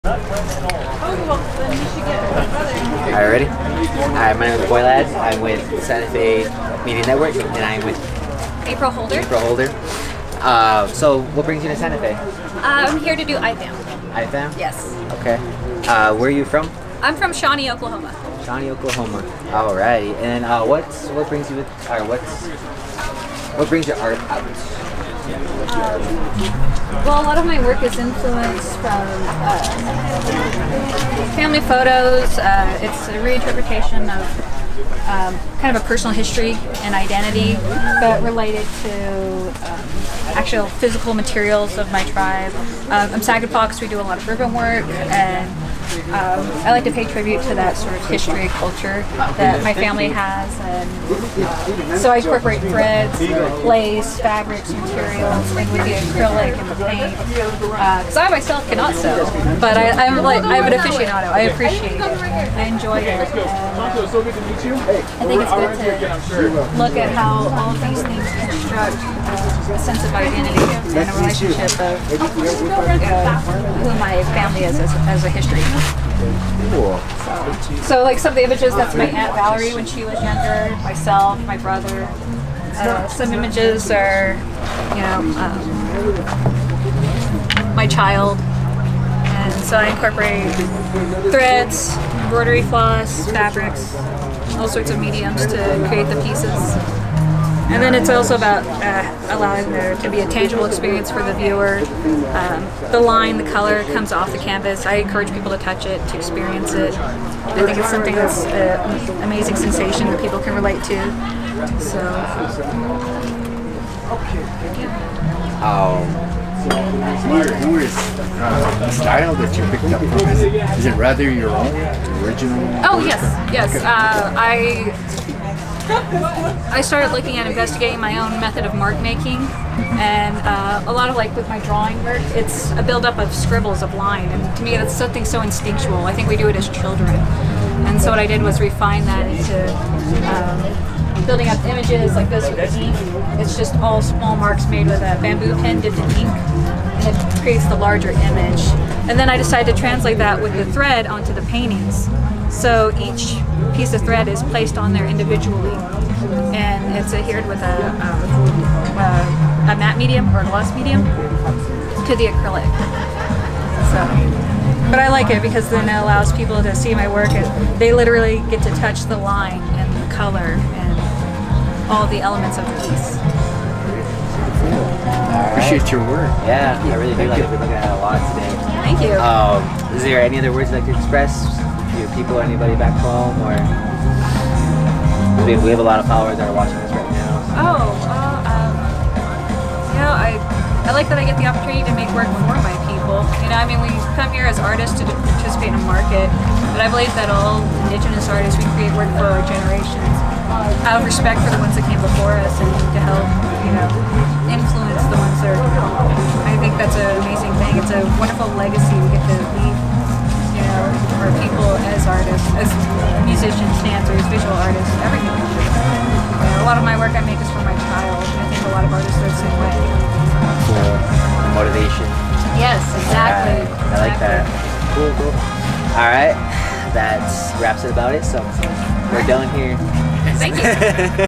Boye Ladd Jr Interviews April Holder - Sac and Fox | Indigenous FIne Art Market
April Holder